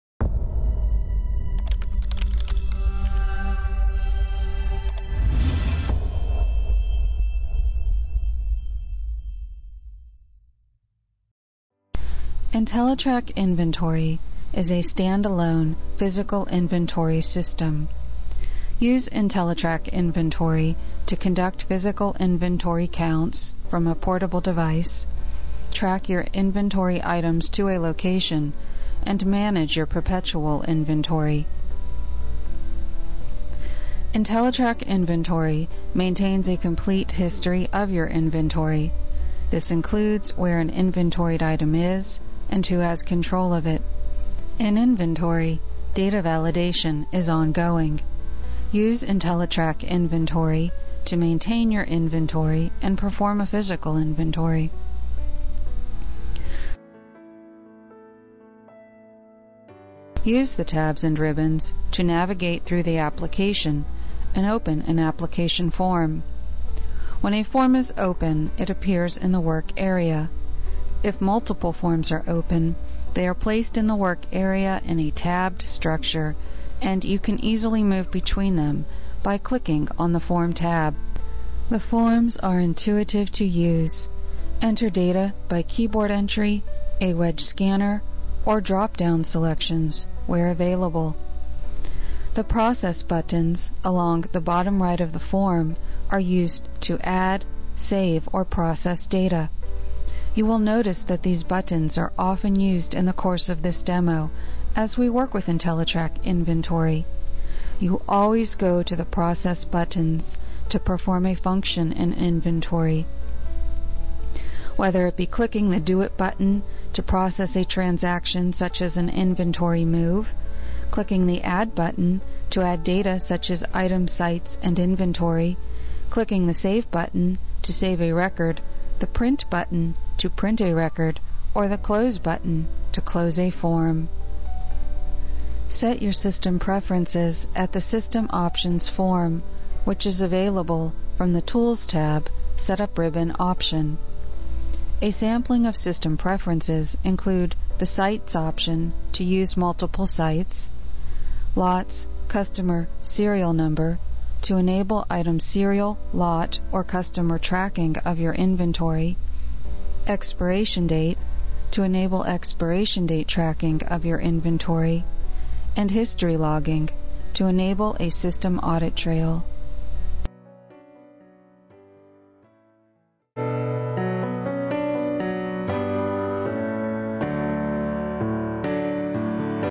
IntelliTrack Inventory Management Software - 1-888-583-3008
IntelliTrack Inventory Management software is a solution for businesses that aren't satisfied with poor inventory management software systems and who want to be the best.

IntelliTrack is the leader in warehouse and inventory software solutions for manufacturers and companies with small, medium and large inventories to track and maintain. IntelliTrack also integrates with Quickbooks. This makes it a perfect problem solver for all sizes of businesses who want powerful inventory tools.

Get rid of duplicate data, old paper based inventory management, extra surplus inventory, and other issues that keep your business from perfoming at its highest potential.

Use IntelliTrack's advanced warehouse inventory management software to reach new company goals and grow your business!